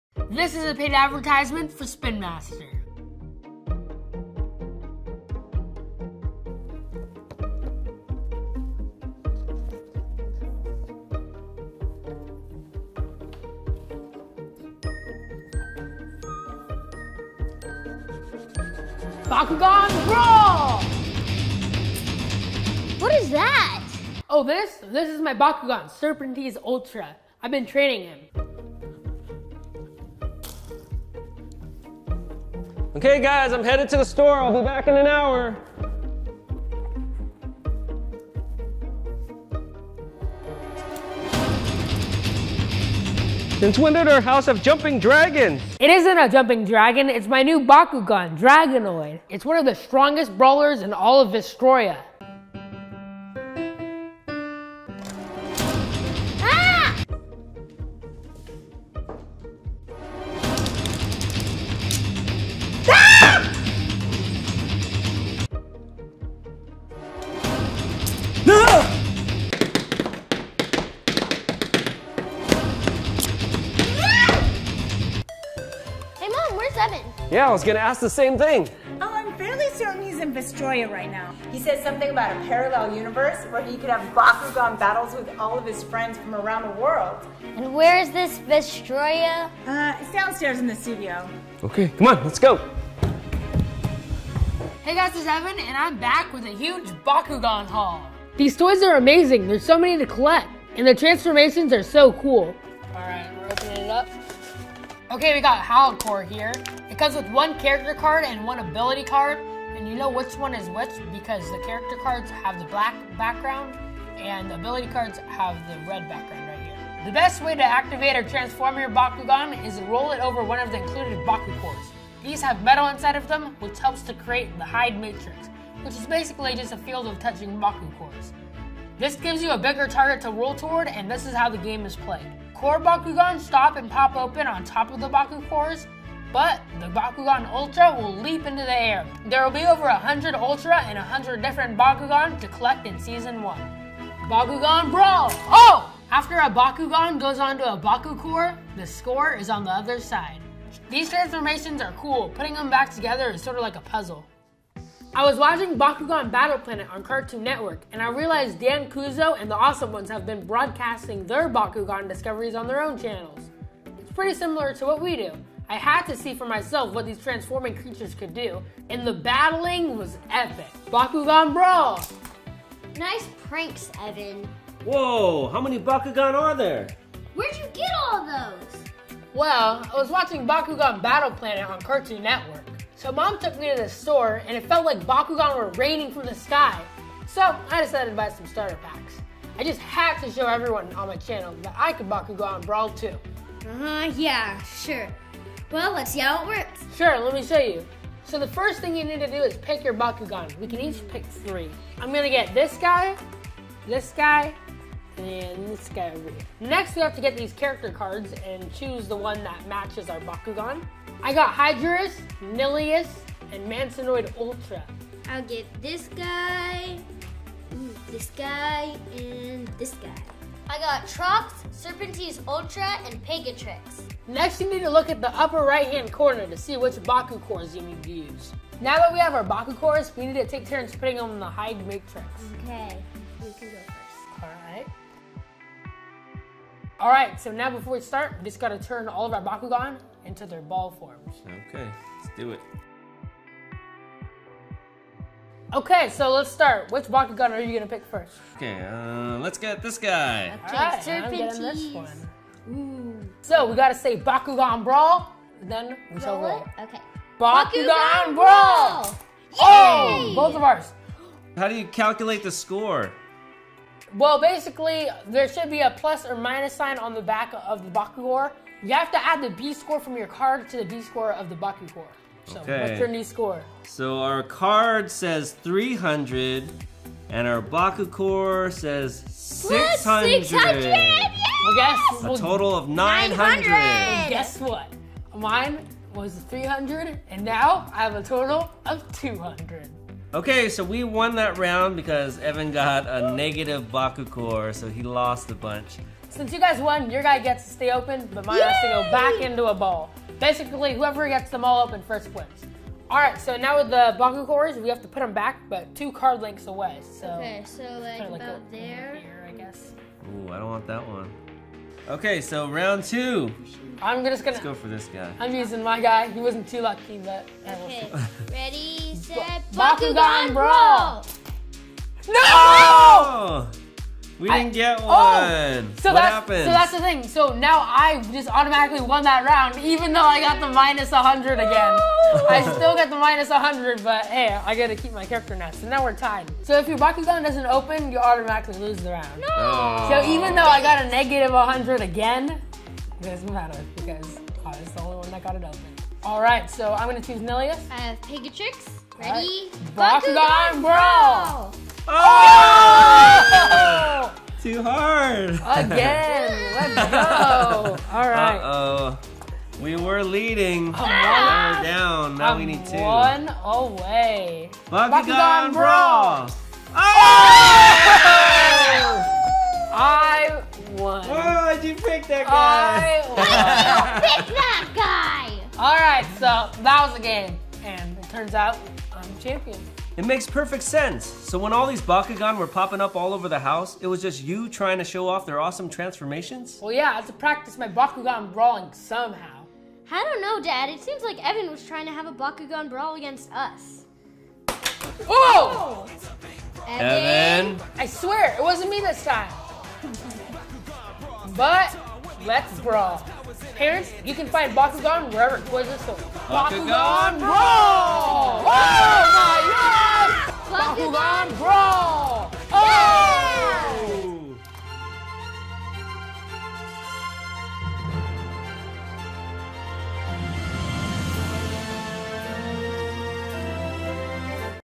BAKUGAN BRAWL!!! EVAN PRANKS HIS FAMILY INTO BATTLE!
Evan uses Bakugan to prank DaddyTube and Jillian! Become a Bakugan Master and answer the call to BakuganBrawl by finding Bakugan everywhere toys are sold! More about Bakugan This is a paid advertisement for Spin Master.